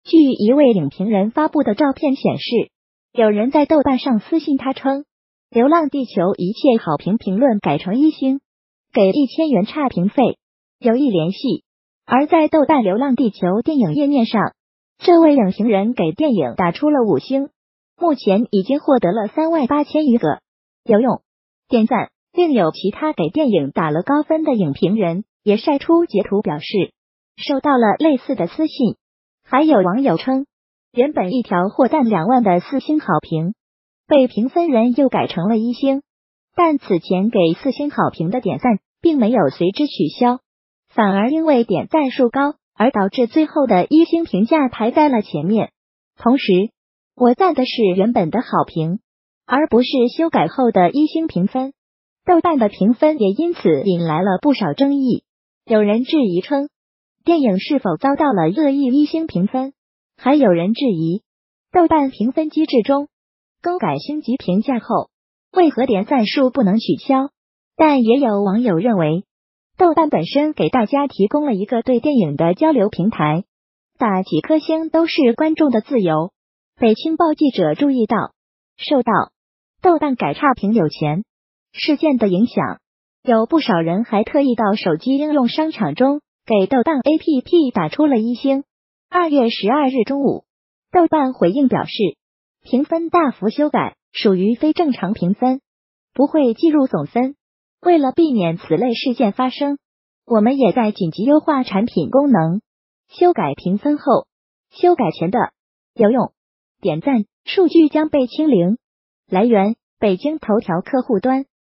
流浪地球被曝有人“收钱改低分”豆瓣紧急优化功能
片显示，有人在豆瓣上私信他称，“流浪地球一切好评评论改成一星，给1000元差评费，有意联系。”而在豆瓣《流浪地球》电影页面上，这位影评人给电影打出了五星，目前已经获得了3万8千余个“有用”（点赞）。另有其他给电影打了高分的影评人也晒出截图表示，受到了类似的私信。还有网友称，原本一条获赞2万的四星好评，被评分人又改成了一星，但此前给四星好评的点赞并没有随之取消，反而因为点赞数高而导致最后的一星评价排